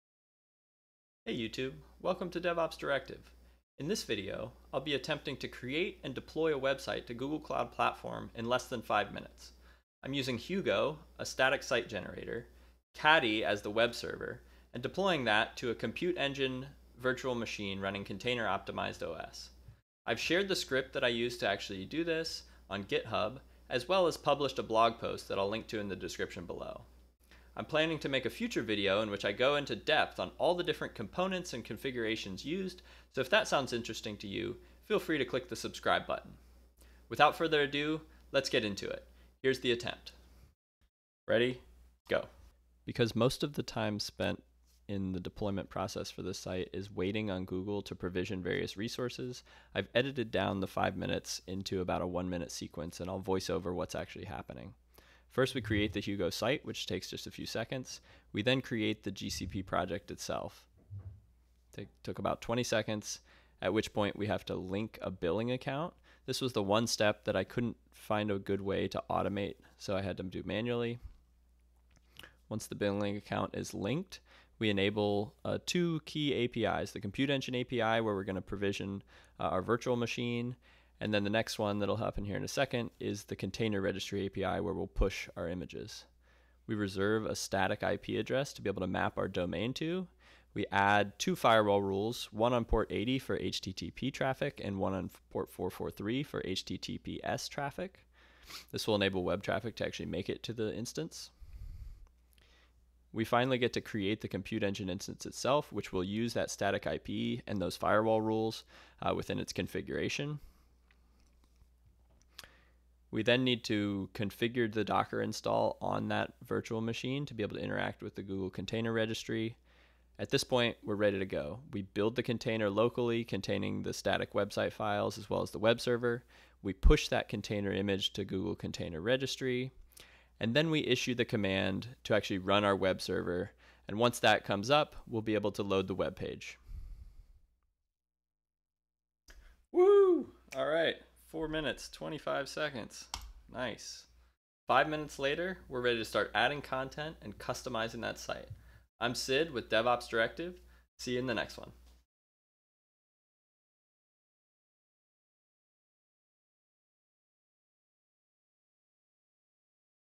Creating and Deploying a Website on GCP in 5 MINUTES for FREE! (Hugo Website + Caddy Webserver)
In this video, I create and deploy a website running in the cloud in less than 5 minutes, including the time to set up and configure a new Google Cloud Platform project!

This setup is the exact configuration I am using (as of February 2020) to run

- Caddy -- web server with automatic HTTPS configuration
 - Containers -- to bundle site and server for deployment
 - Google Cloud Platform -- the webserver is running on a Compute Engine virtual machine

I also wrote an article which has a much more in-depth explanation of the setup process

This GitHub repository contains all of the commands used and a walkthrough